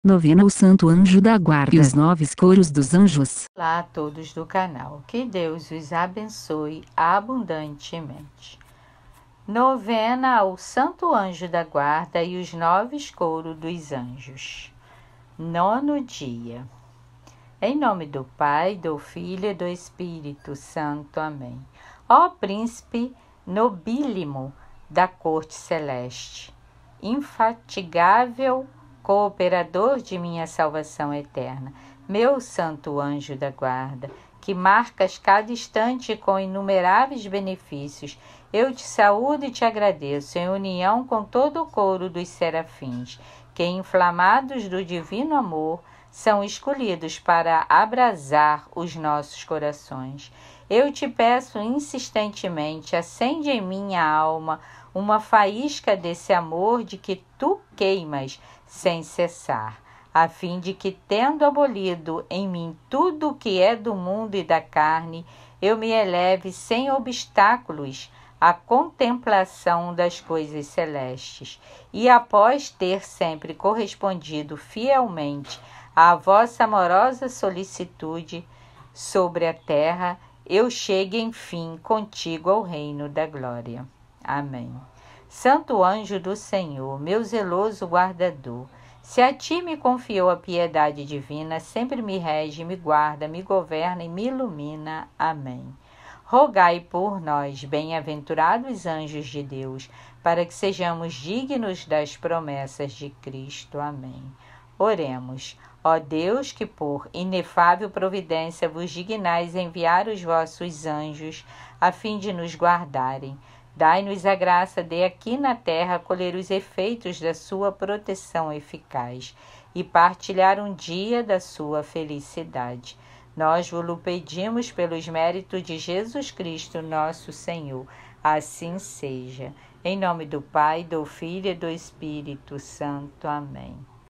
NOVENA AO SANTO ANJO DA GUARDA E OS NOVES COROS DOS ANJOS
NONO DIA:

Ó príncipe nobílimo da corte celeste, infatigável cooperador de minha salvação eterna, meu Santo Anjo da Guarda, que marcas cada instante com inumeráveis benefícios: eu te saúdo e te agradeço, em união com todo o coro dos Serafins, que, inflamados do divino amor, são escolhidos para abrasar os nossos corações. Eu te peço insistentemente: acende em minha alma uma faísca desse amor de que tu queimas sem cessar, a fim de que, tendo abolido em mim tudo que é do mundo e da carne, eu me eleve sem obstáculos à contemplação das coisas celestes, e, após ter sempre correspondido fielmente à vossa amorosa solicitude sobre a terra, eu chegue enfim, contigo, ao Reino da Glória.

Santo Anjo do Senhor, meu zeloso guardador, se a ti me confiou a Piedade Divina, sempre me rege, guarda, governa e ilumina. Amém.

V) Rogai por nós, Bem-Aventurados Anjos de Deus,
R) Para que sejamos dignos das promessas de Cristo.

Oremos:
Ó Deus, que, por inefável Providência vos dignais enviar os vossos Anjos a fim de nos guardarem, dai-nos a graça de aqui na terra colher os efeitos da sua proteção eficaz e partilhar um dia da sua felicidade; nós vo-lo pedimos pelos méritos de Jesus Cristo, nosso Senhor. Assim seja.